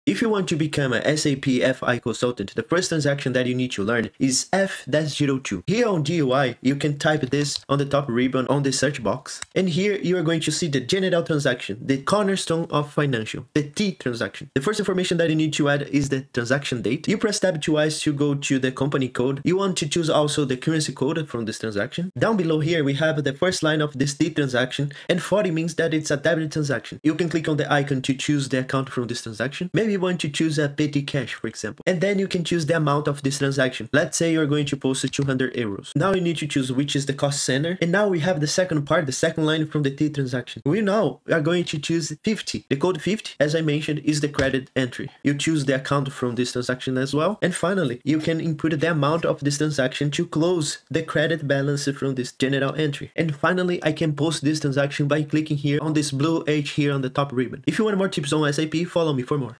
Master SAP F-02: The First Transaction Every SAP FI User Must Learn!
New to SAP Financial Accounting (FI)? The first transaction every SAP FI user should learn is F-02 (GL Account Posting)! In this tutorial, I’ll show you how to post financial entries in SAP, ensuring accurate and efficient general ledger accounting.

In this video, you’ll learn:
✔️ What F-02 does and why it’s important
✔️ Step-by-step GL account posting in SAP
✔️ Best practices for financial data accuracy
✔️ How mastering F-02 helps build a strong SAP FI foundation

This is the first step to becoming an SAP Finance expert!